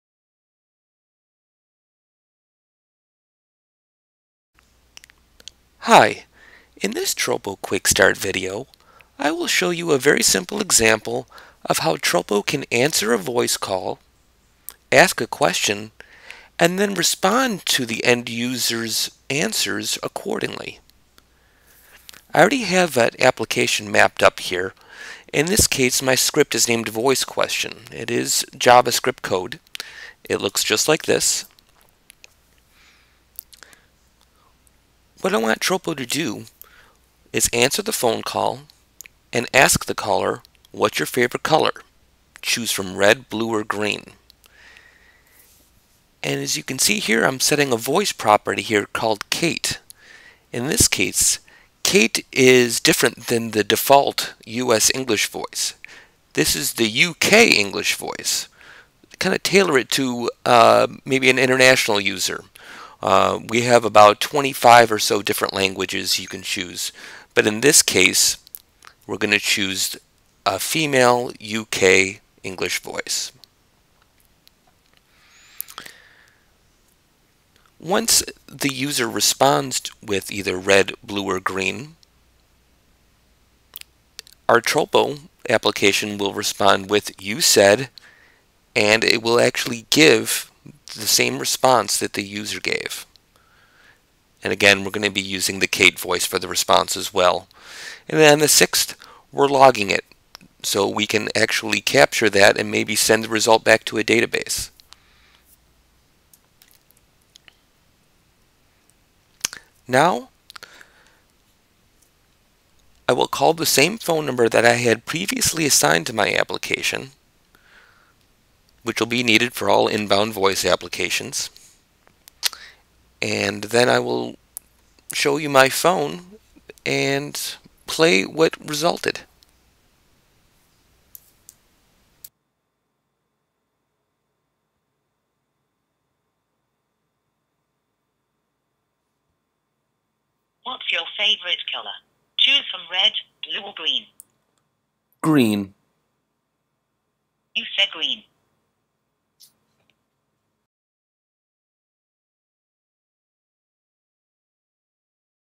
Tropo- quick start - answer voice call
This 2 minute Quick Start shows you how to get your phone to answer a voice call, ask a question and then respond accordingly.  It's all done using Tropo - a cloud API that enables you to automate communications within your web-based and mobile applications.